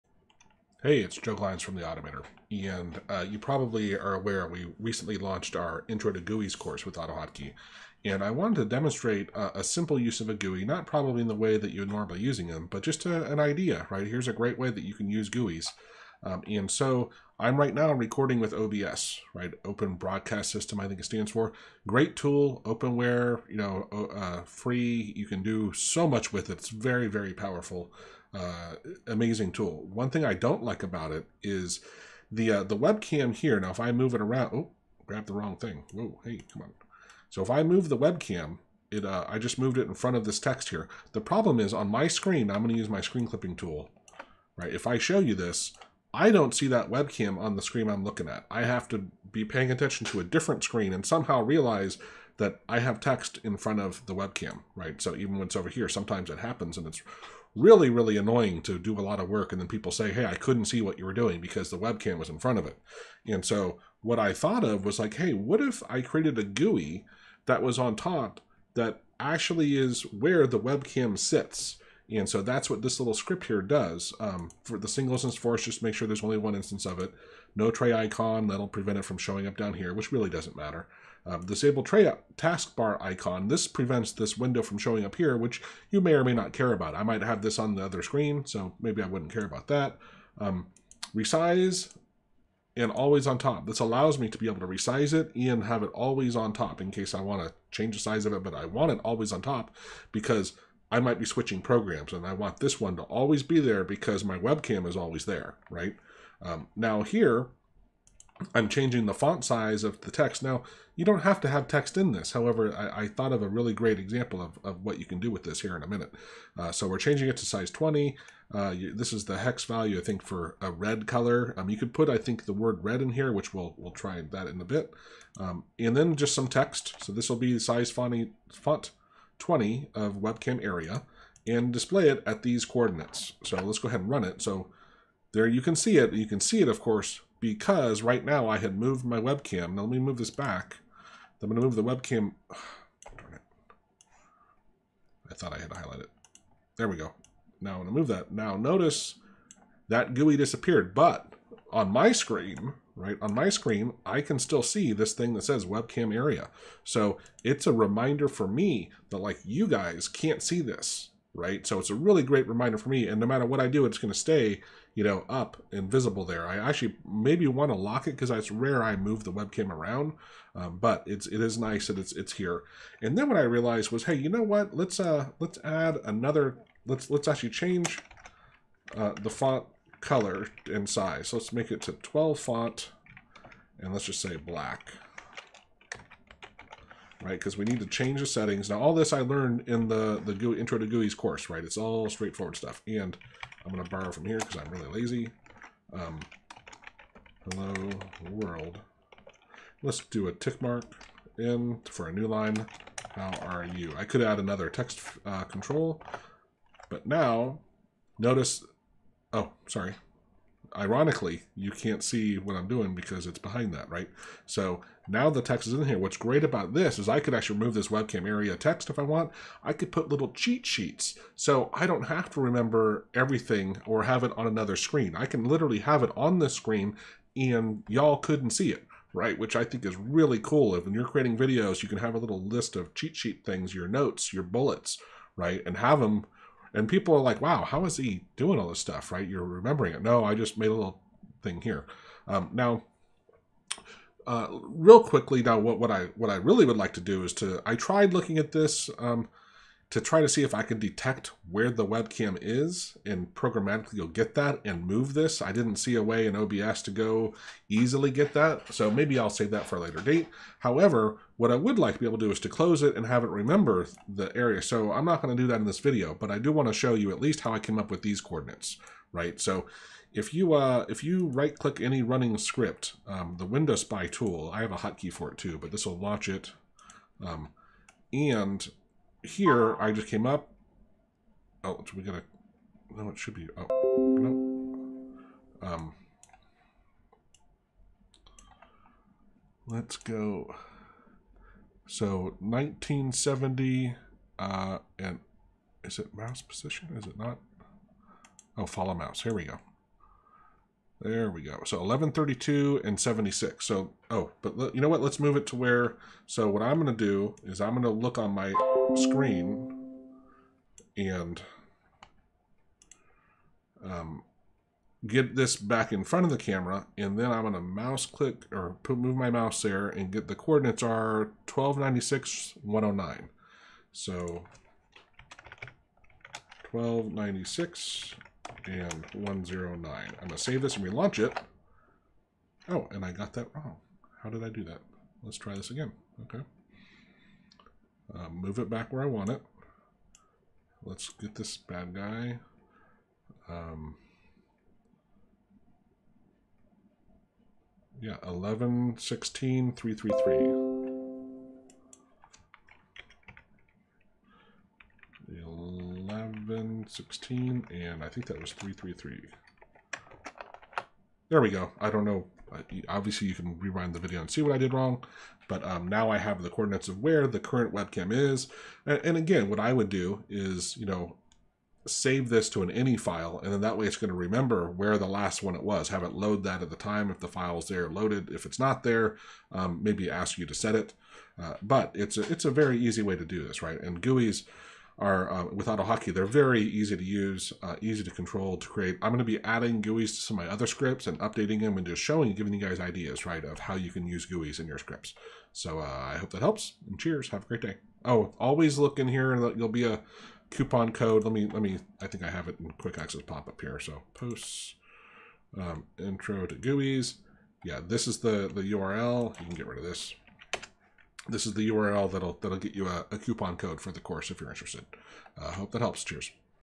🚨 Don't Let Your Webcam Ruin Your SCreen Recording: Learn This Hack!
In this AutoHotkey Tutorial I show how to never forget your webcam is covering the screen by creating a simple GUI with AutoHotkey.  GUIs are easy with AutoHotkey and I show you how to
create a reminder where your webcam is while recording in OBS (but it will work for any program you're recording with)

Adding a simple GUI with AutoHotkey to remind me where the Webcam is during recording is a great, easy, way to avoid huge mistakes with OBS.  OBS is a great tool however it's really easy to forget that your webcam is covering your screen and that people viewing the recording will not be able to see it.  I use OBS for recording my screen but often forget where my Webcam is (and viewers can't see behind it).  This simple script helps me remember where my webcam is.

In this AutoHotkey GUI tutorial I demonstrate some of the code I learned from watching our Intro to GUIs with AutoHotkey Course.    This simple GUI creates a visual reminder for me to know where my webcam is.  It also allows me to have notes on the screen that viewers can't see.  :)

; Webcam Reminder GUI Code
SingleInstance,Force ;only have 1 instance
NoTrayIcon ;not showing in the system tray
gui,+owner ; disable taskbar icon
gui,+Resize +AlwaysOnTop
gui,font,S20 C0xff0000 ;Change size of font and set color to red
gui,Add,Text,,Webcam Area
gui,font,S12 Black
gui,Add,Text,,Hello World.`nHow are you
gui,Show,x1116 y333 w345 h232
return